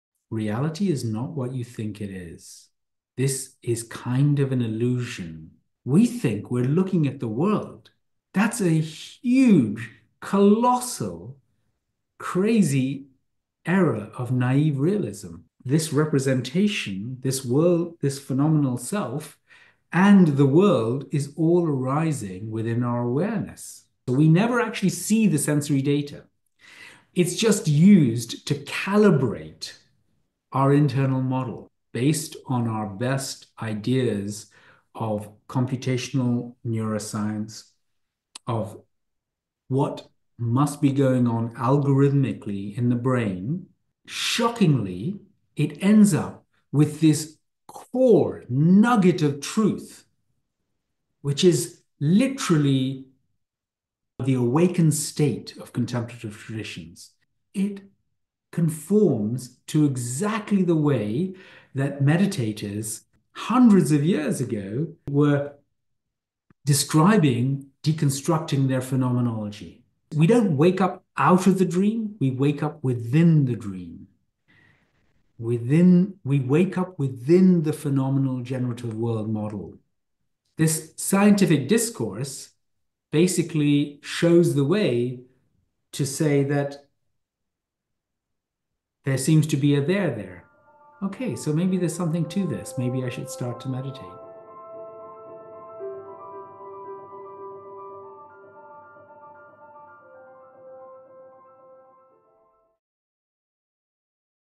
Neuroscience of meditation & awakening - Shamil Chandaria
Dr Shamil Chandaria is a senior research fellow at the centre for Eudaimonia and Human Flourishing, University of Oxford. He is also a research fellow at the Institute of Philosophy, London University, where he focuses on philosophical issues in neuroscience and well-being;  a research fellow at Imperial College in the neuroscience of psychedelics; and a bye-fellow at Fitzwilliam College, Cambridge University.

He has multi-disciplinary research interests spanning computational neuroscience, machine learning & artificial intelligence, and the philosophy and science of human well-being. He is currently working on computational theories of consciousness and meditation.  Shamil has been a meditator for 38 years and has contemplative training in early Buddhism, Tibetan Buddhism, Advaita Vedanta and Kashmir Shivaism.